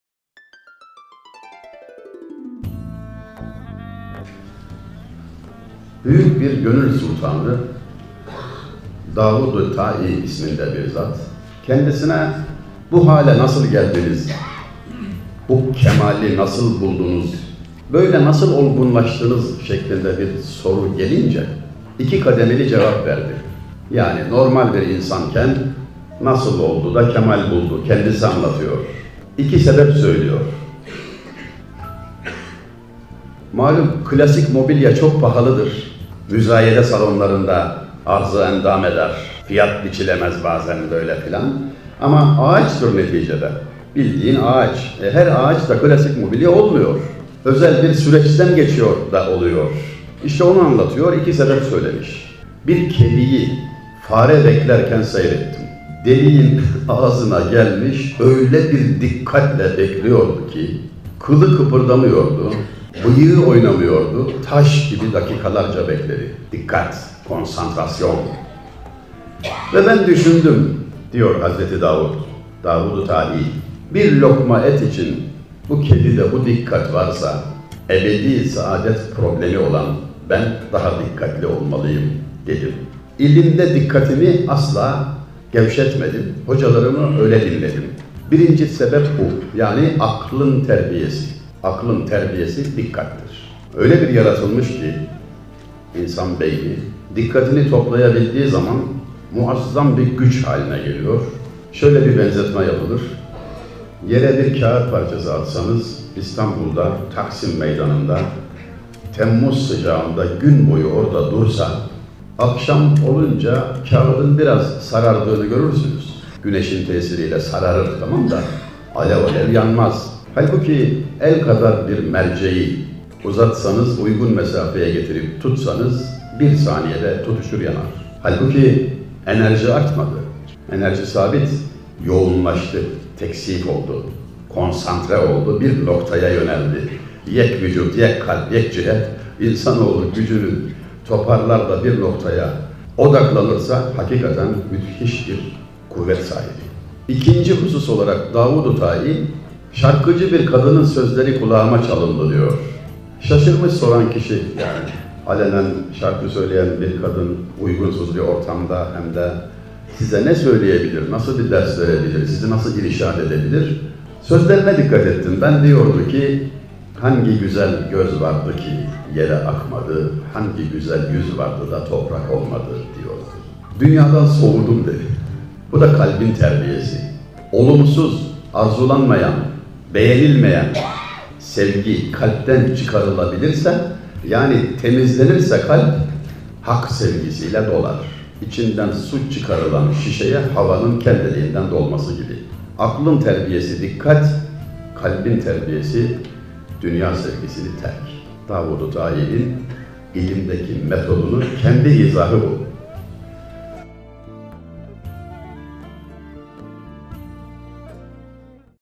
Hayati İNANÇ | Dâvûd-i Tâî'nin İlim Metodu | BAİHL
Okulumuz Beyoğlu Anadolu İmam Hatip Lisesi'nde Veli Akademileri projesi kapsamında konuk ettiğimiz Hayati İNANÇ hocamızın programından bir kıssa.